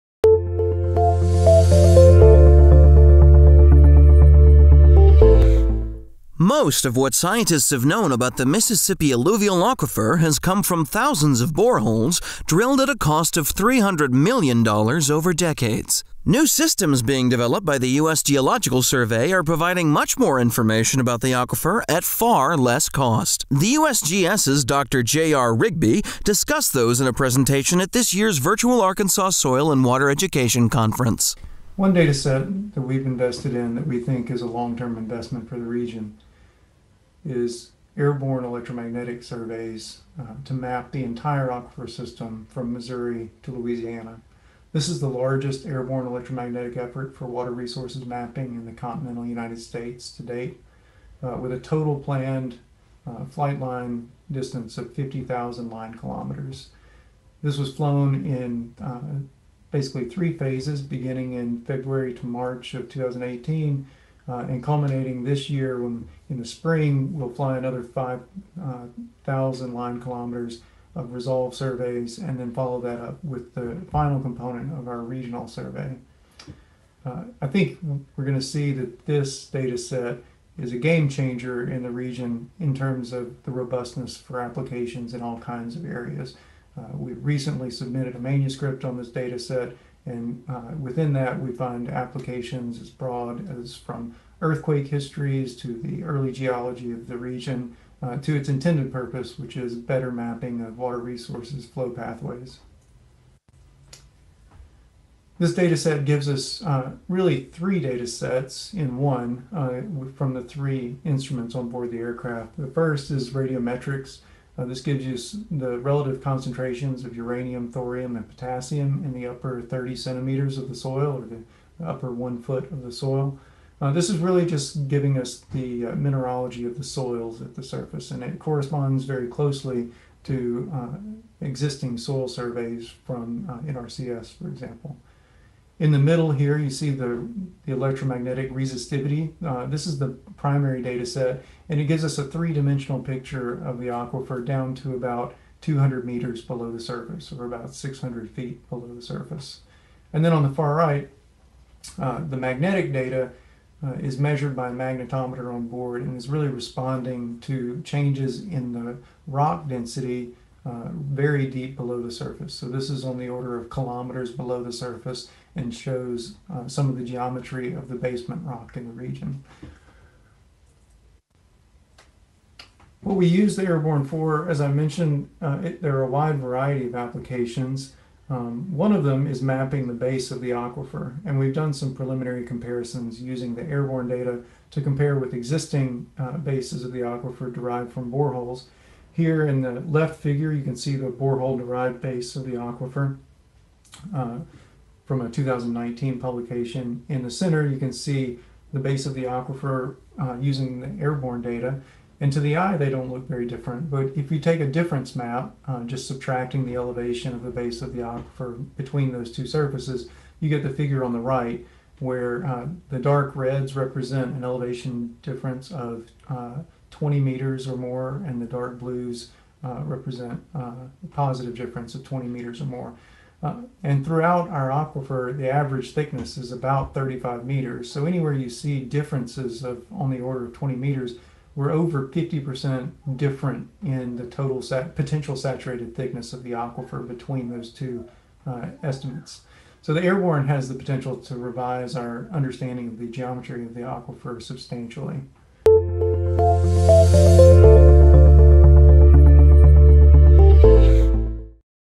Mississippi Alluvial Aquifer
USGS and USDA’s ARS are using new tools to explore the Mississippi Alluvial Plain.